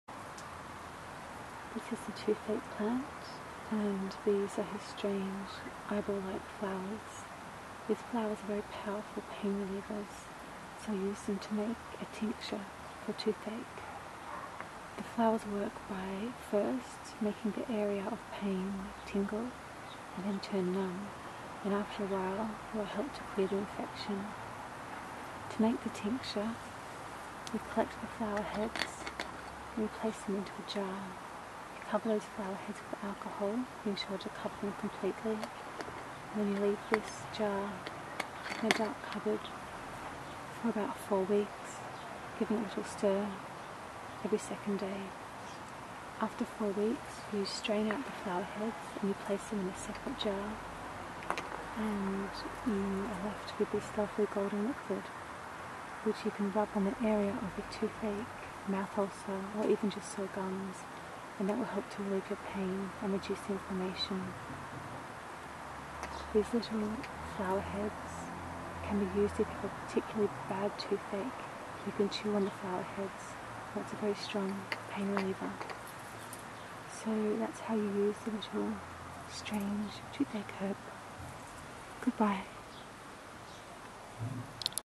Natural Pain Relief: How To Make A Local Anaesthetic Tincture For Toothache, Sore Gums, Mouth Ulcers
The toothache plant is an extremely effective & powerful pain relieving herb, here I explain how to extract the analgesic constituents of Acmella oleracea. This plant goes by many names, Jambu, Electric Daisy, Buzz buttons & Paracress. Acmella oleracea's leaves and flowers contain spilanthol, a chemical with a strong local anaesthetic action that offers us relieve from pain. When tinctured  toothache plant is rubbed on an area of pain in the mouth you will feel a tingling, buzzing sensation followed by a cooling numbness. Meanwhile, the plant is working on a deeper level helping to clear infection and strengthen the gums.

Goodbye,
Plants & Creatures

Plants and creatures: Acmella oleracea

Please thoroughly research, any plant you wish to use. It is wise to consult at least three reliable sources, preferably botanical books each time you consider utilising a new plant, this will help to familiarise yourself with the particular nature of each individual plant.